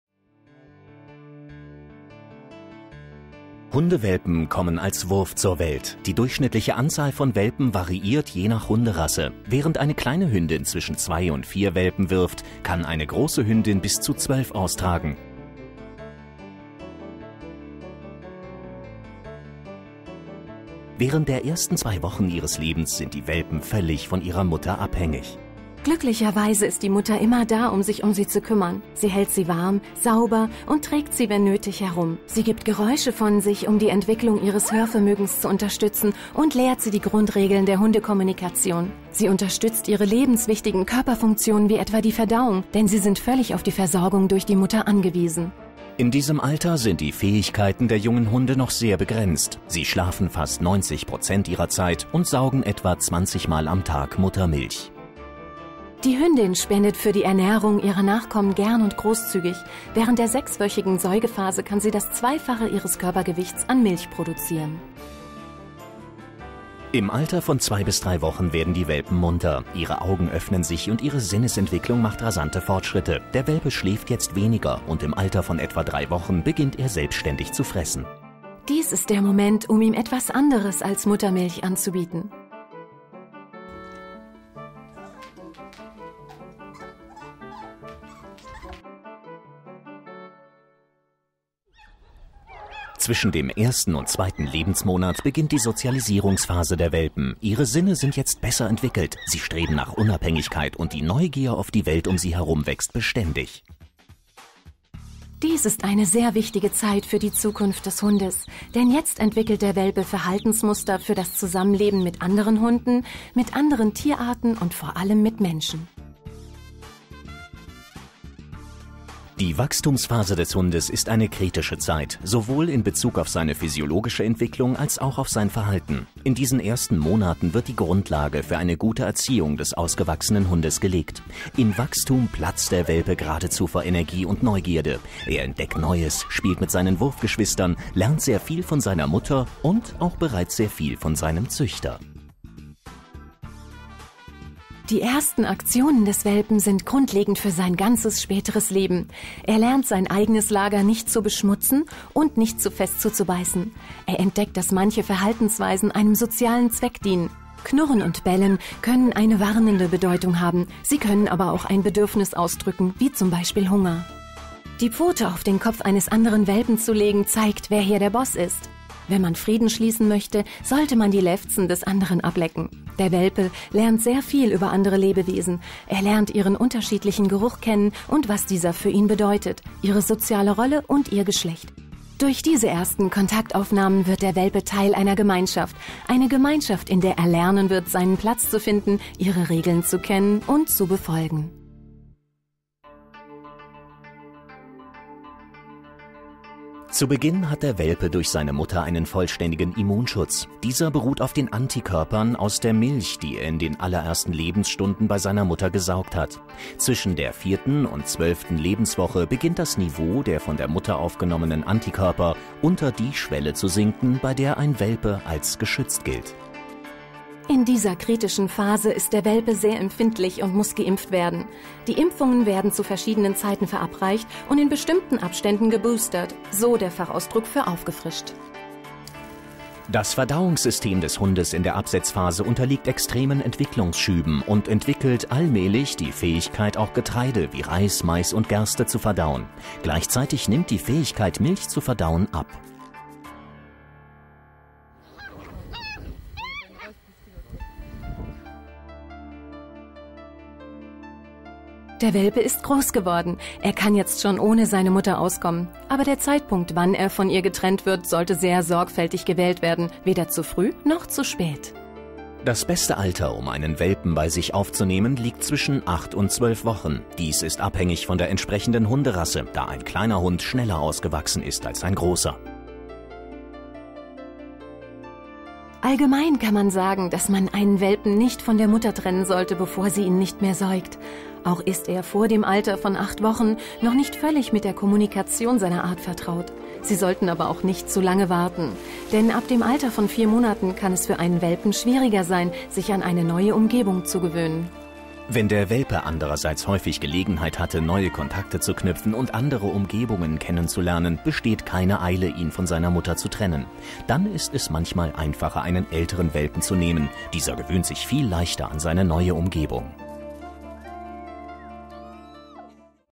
Die Welpenentwicklung im Wurf
Die ersten Schritte nach der Geburt. Der Welpe lernt seine Umwelt kennen. Welche Rolle nimmt die Mutter ein?

Alle Videos zum Thema Hund von ROYAL CANIN finden Sie